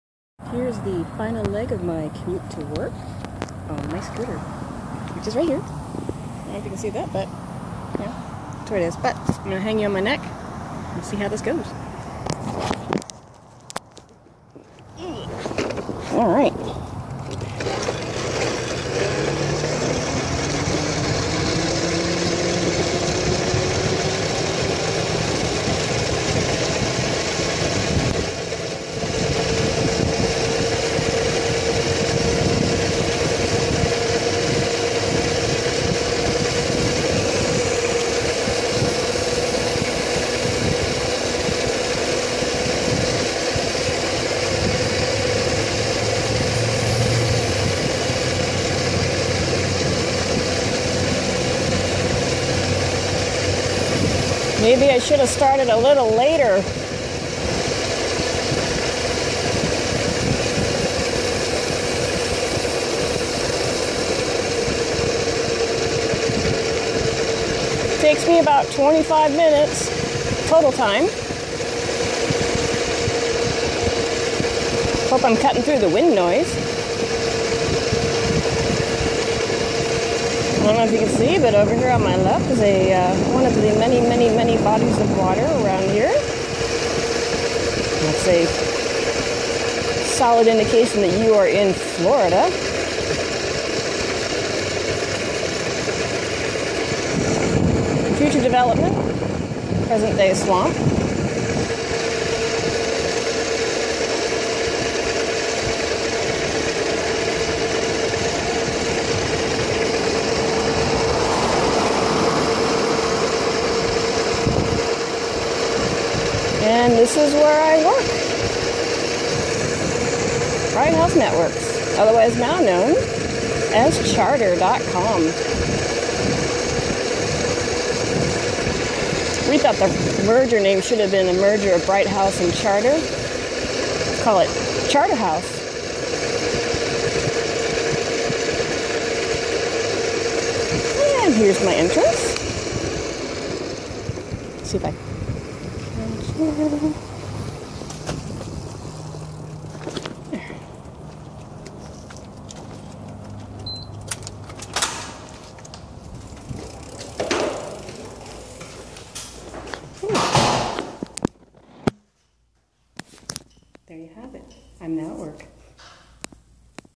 Electric-scooter commute to work
The final few blocks to work, out of a 3.5 mile ride, on my electric scooter.